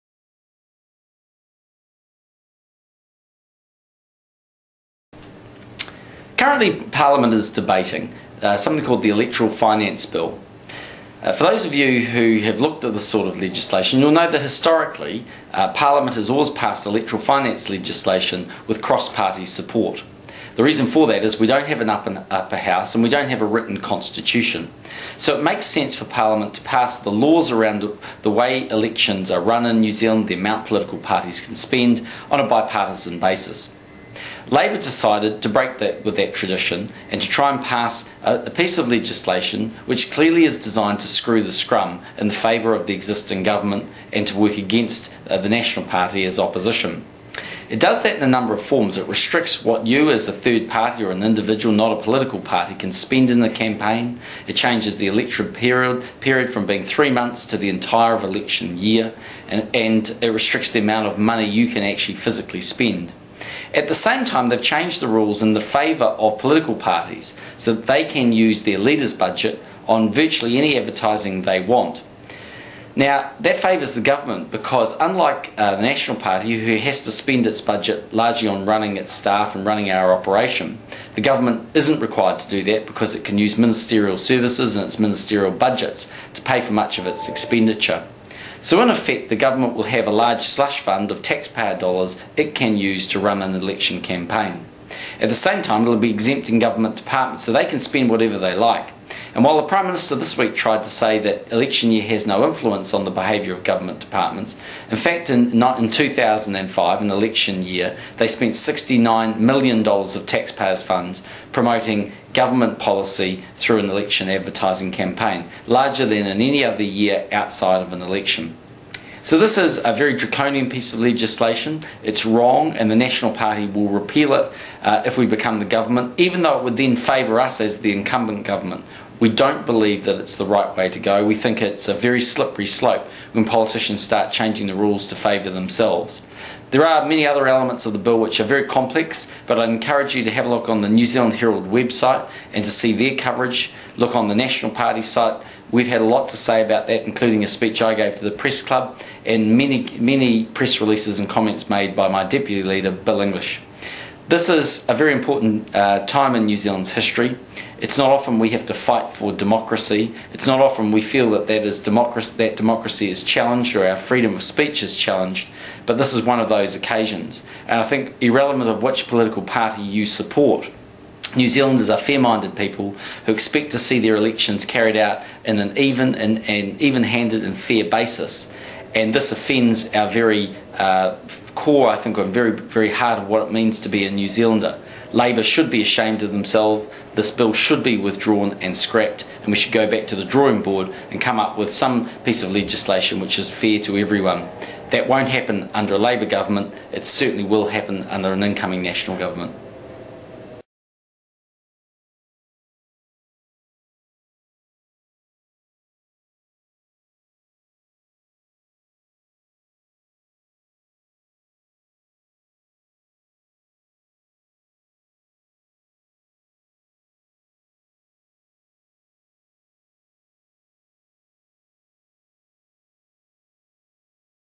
John Key - Video Journal 26 on election finance 'reforms'
13 November. John outlines the most egregious components of the Labour Government's proposed 'reforms' to the manner in which elections are run and reassures viewers that, if elected, National will scrap the legislation.